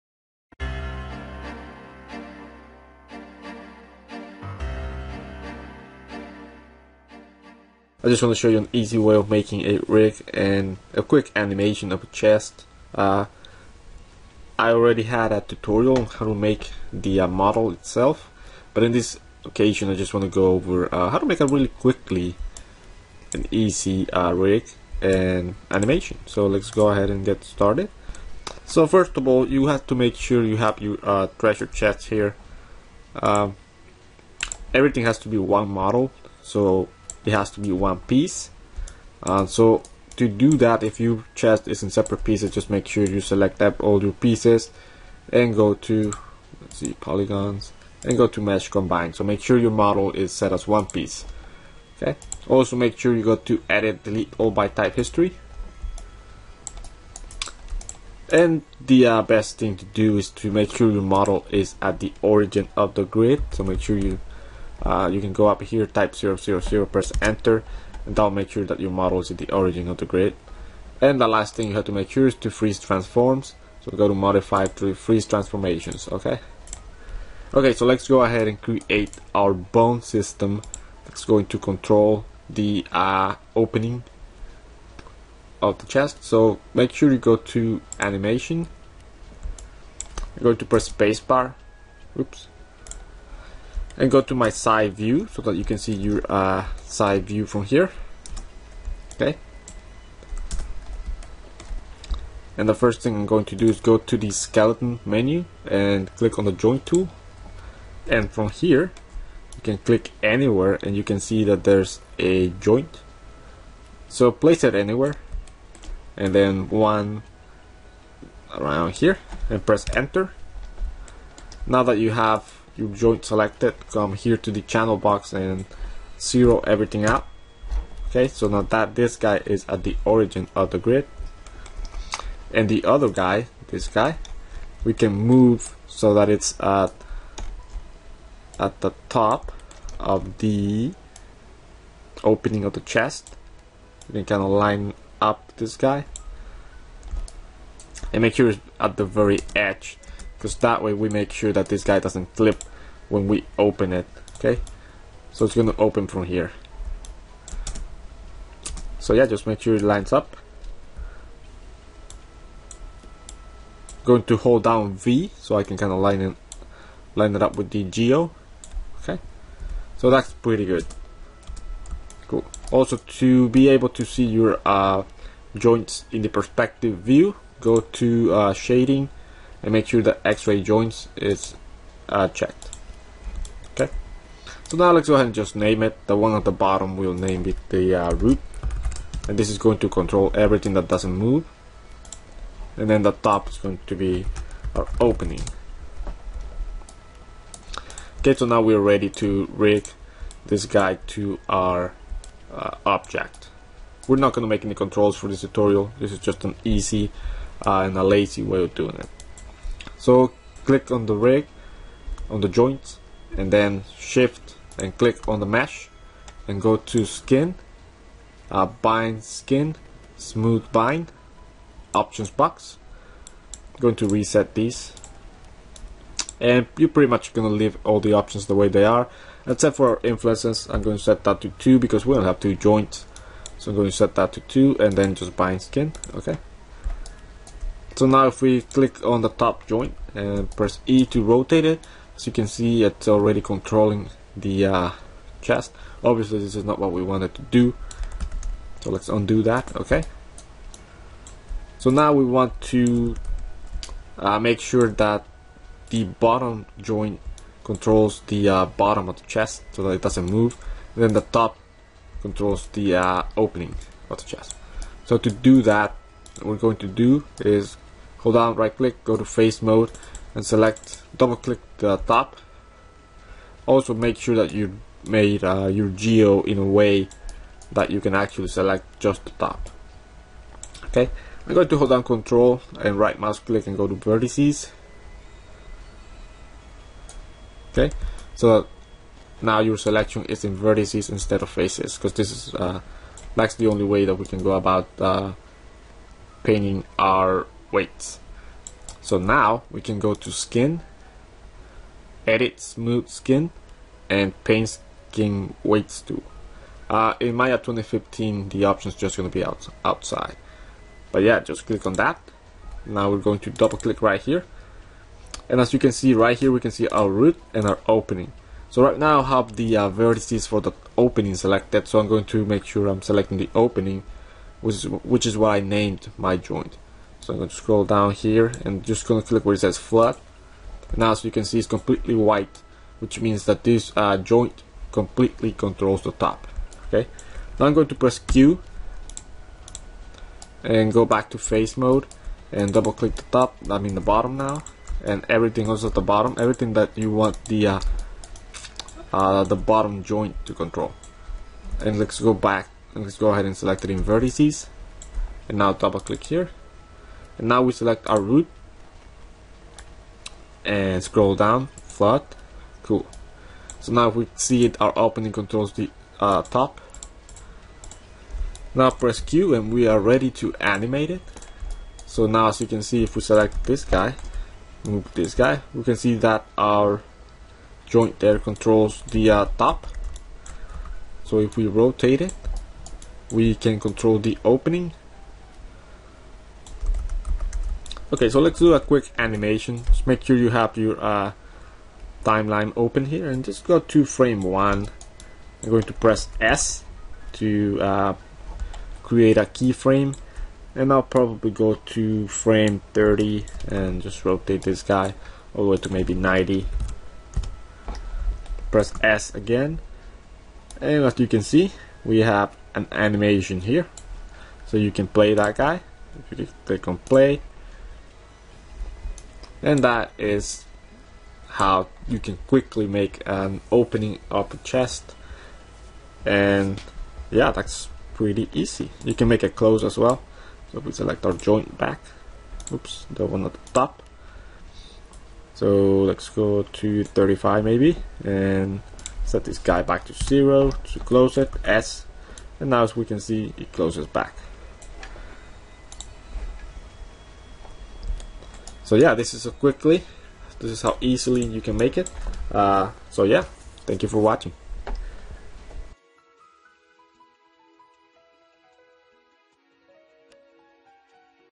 Autodesk Maya Tutorial - Easy Chest Rig and Animation
I quick tutorial showing how to easily make a rig for a chest and using it to create an animation.

Maya Version Used : 2014, but any other version may be used from Maya 2011 to Maya 2015.